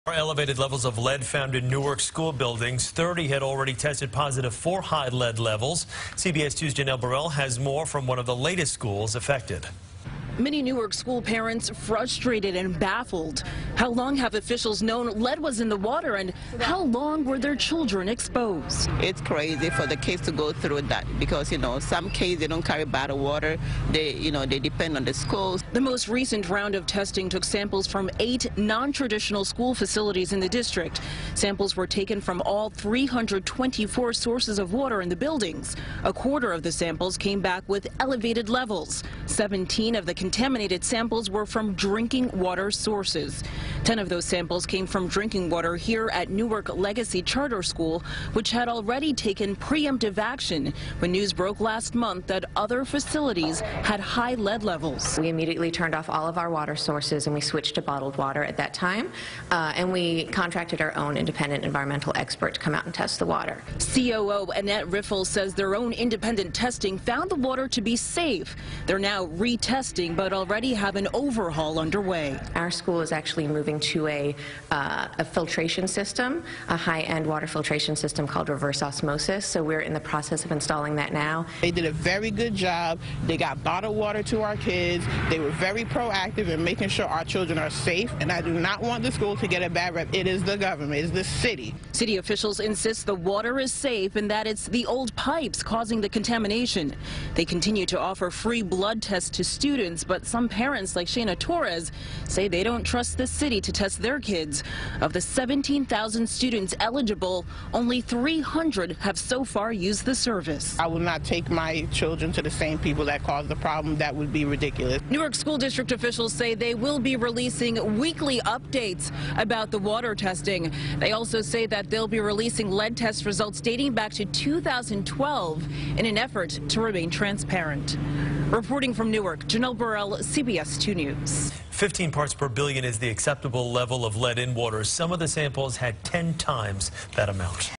Elevated Lead At More Newark Schools
CBS2's Janelle Burrell reports.